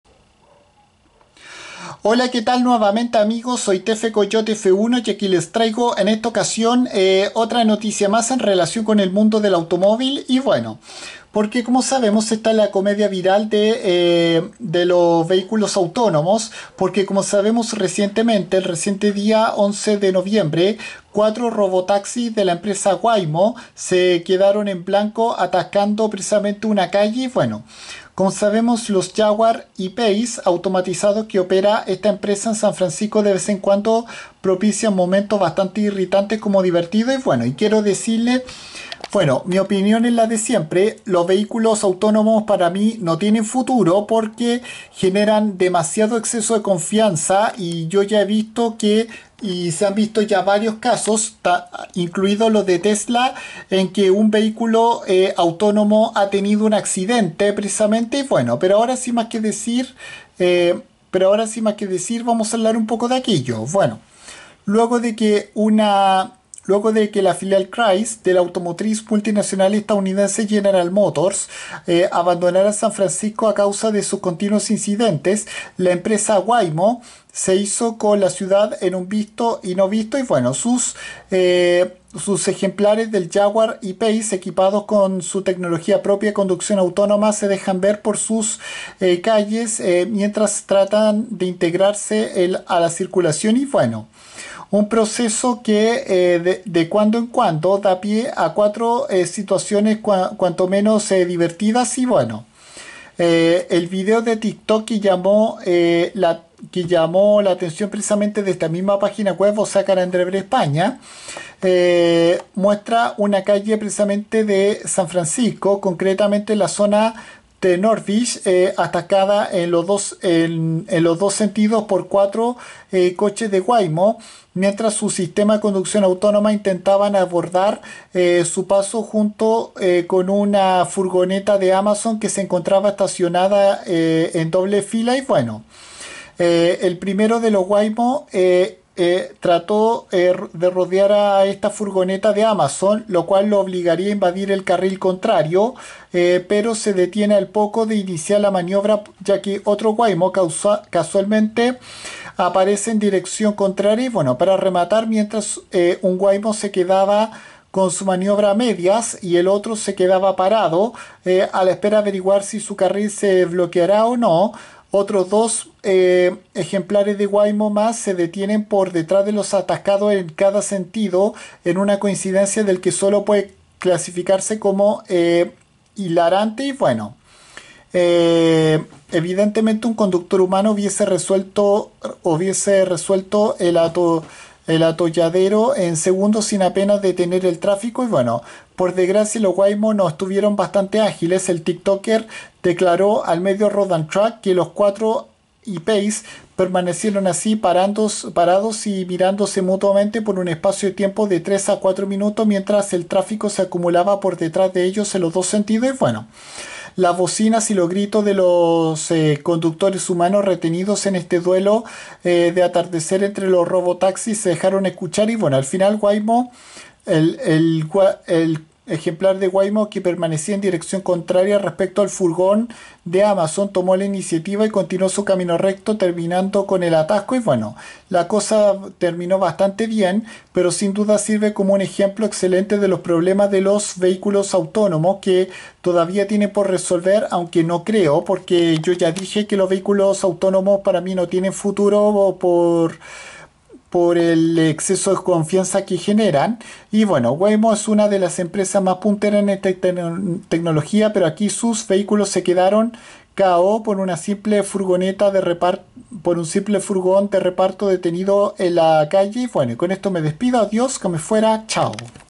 Cuatro robotaxis de la empresa Waymo se quedan en blanco y provocan atascos
Estos eran ejemplares de Jaguar I Pace que provocaron estos mismos incidentes en San Francisco

PD: Los vehículos autónomos para mi no tienen futuro porque causan muchos excesos de confianza que provocan accidentes